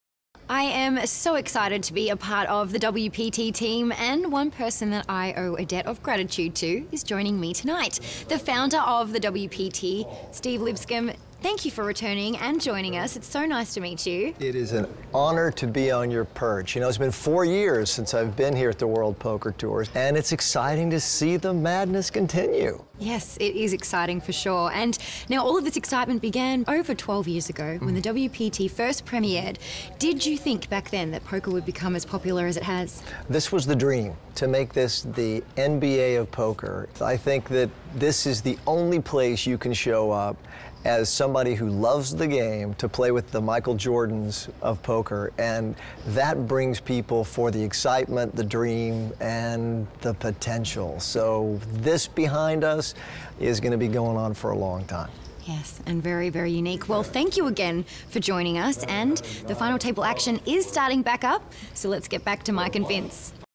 WPT Season 12 Borgata Poker Open: Interview with Steve Lipscomb
The World Poker Tour (WPT) is a series of international poker tournaments and associated television series broadcasting the final table of each tournament.